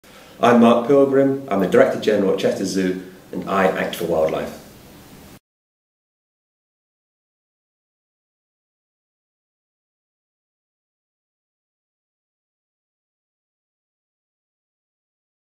I act for wildlife.wmv
Director General of Chester Zoo, Mark Pilgrim Acts for Wildlife